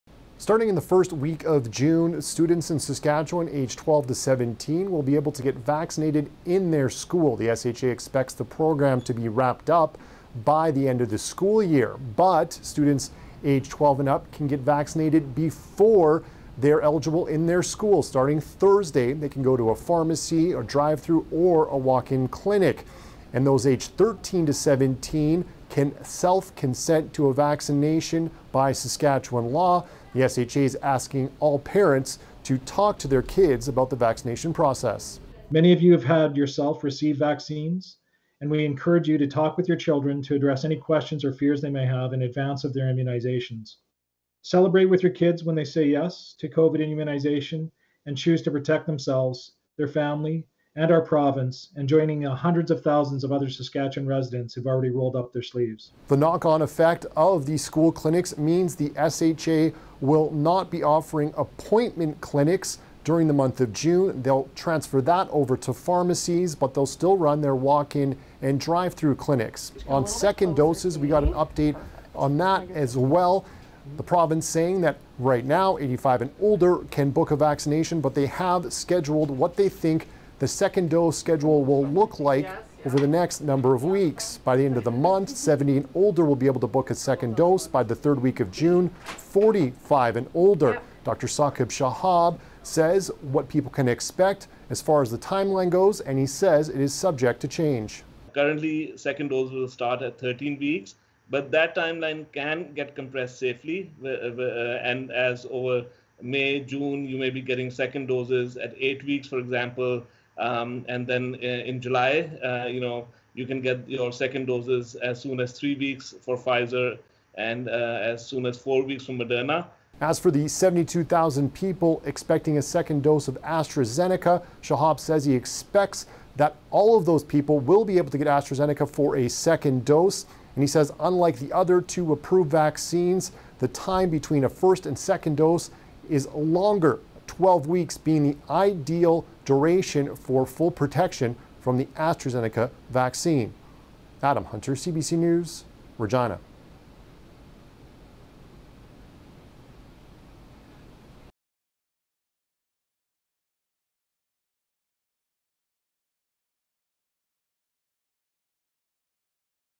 SK government reveals plans for vaccines for kids 12 and up, along with second doses
Sask. top doctor still urges caution as COVID-19 rules due to be relaxed